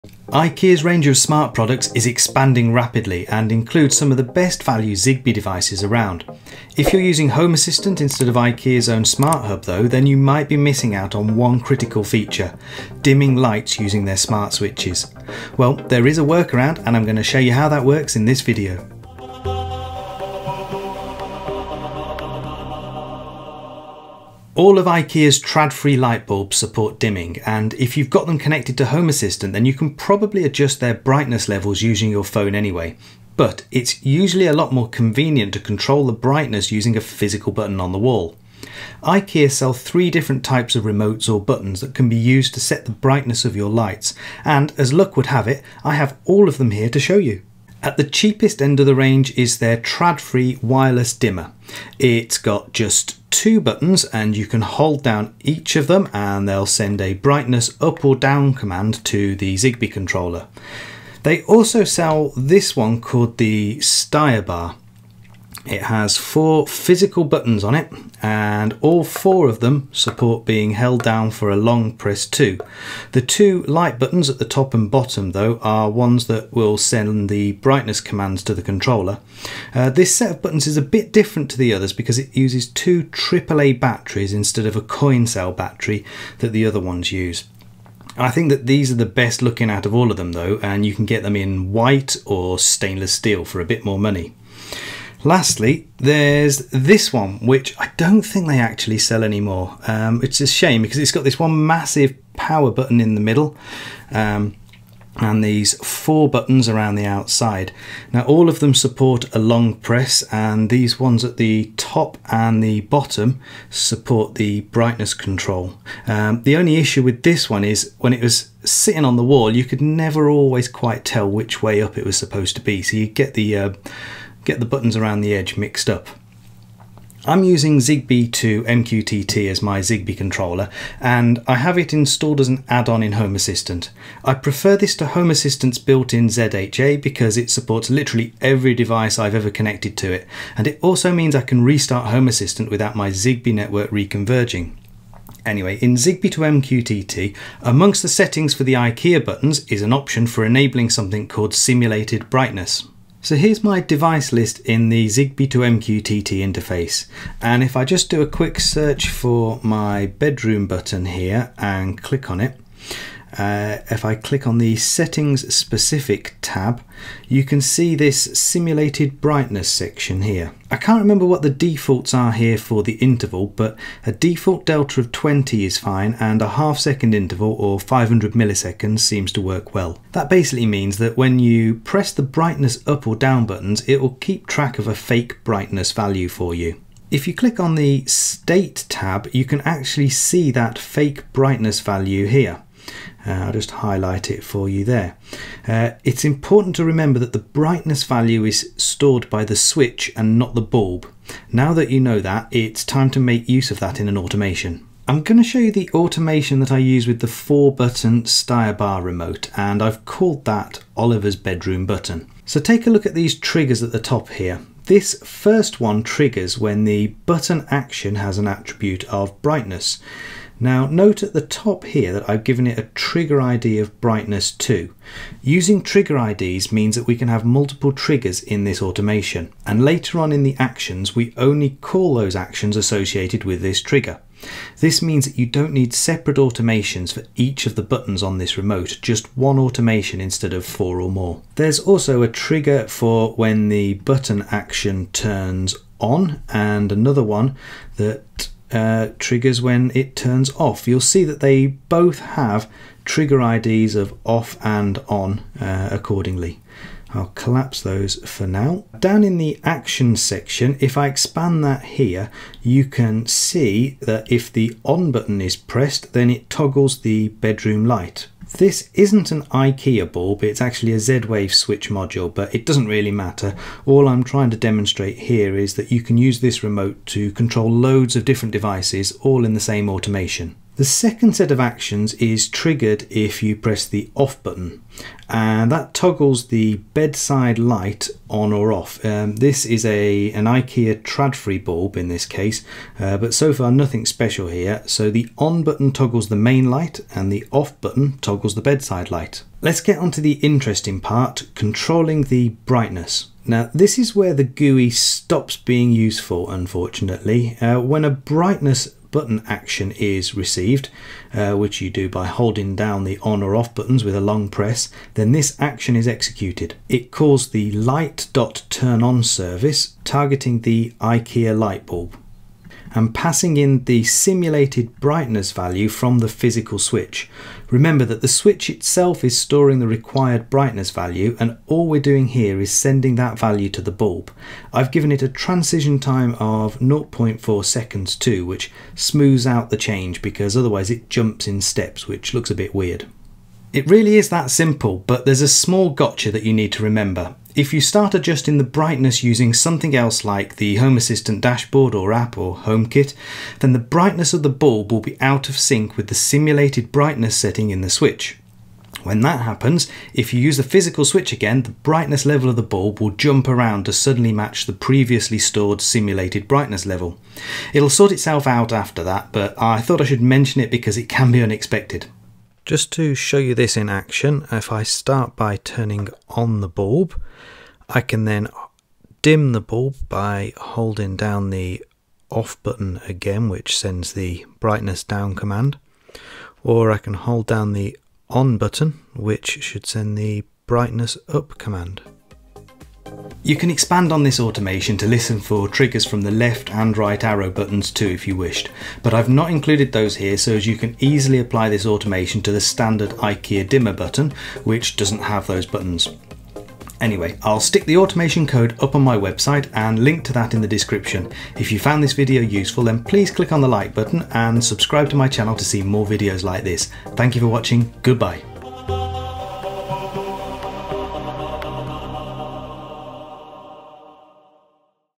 IKEA Tradfri Brightness Control with Home Assistant (zigbee2mqtt)
IKEA's smart remotes can control the brightness of your smart bulbs. If you're using Home Assistant, here's how you configure the simulated brightness feature of zigbee2mqtt and make use of that in an automation.